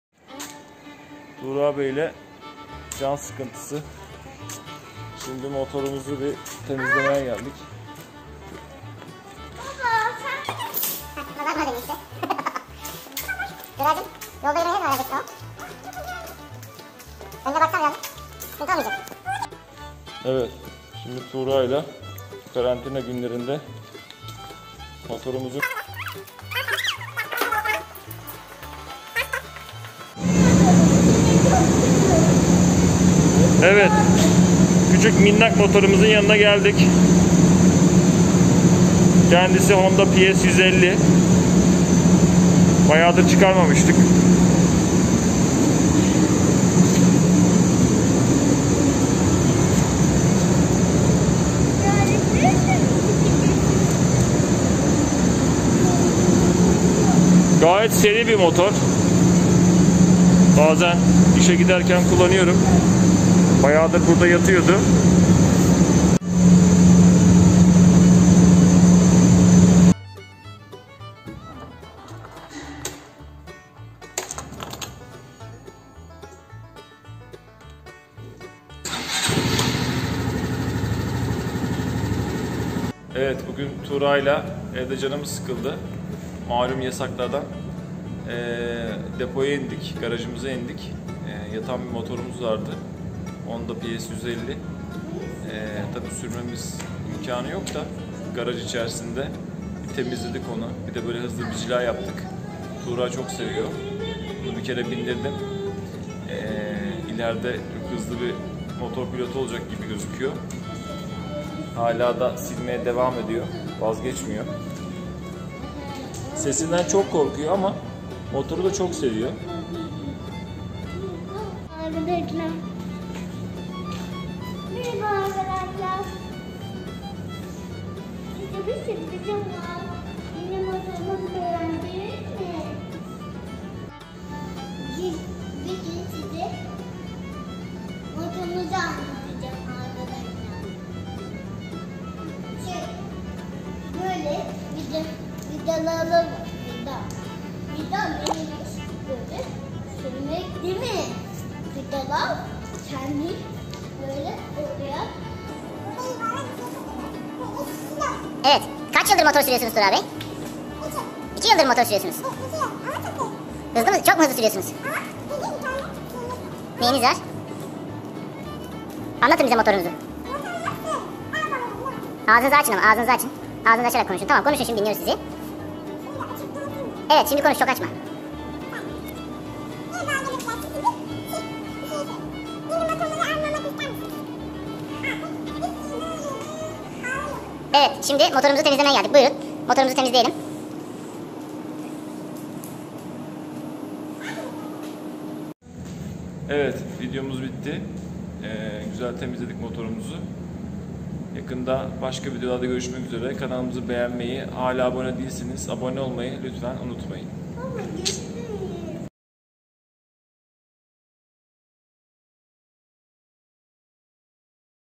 Honda Ps150i Test
honda scooter hondaps150i Karantinada canımız sıkılınca garaja indik ve motorumuzu yıkadık parlattık,honda motosiklet,
2020 honda motosiklet modelleri,
2020 honda motosiklet fuarı,
2020 honda motosiklet,
honda 4 silindir motosiklet,
honda motosiklet 50 cc